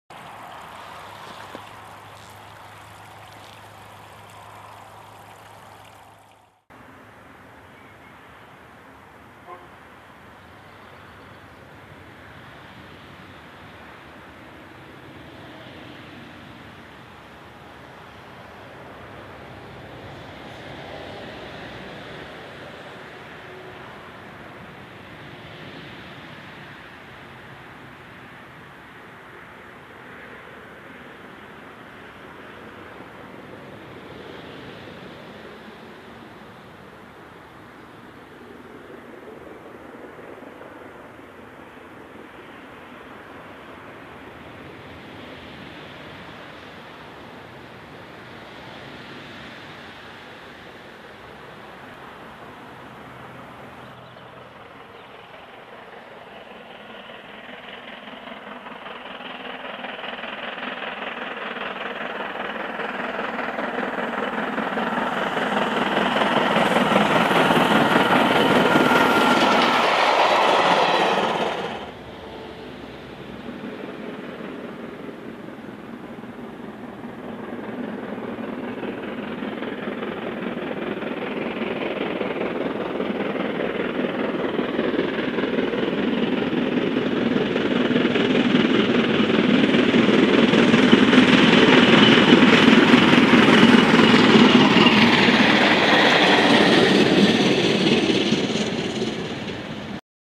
LMS 45305 on a engine move from Carnforth to Edinburgh on 21-4-12 .
LMS Black 5 45305 moved with support coach from Carnforth to Edinburgh in readiness for day 3 of the Great Britain 5 tour of Britain. The 5 left early without 46115 as planned and is seen storming up Shap at Scout Green . Thanks to Stu for the heads up about the change of plan.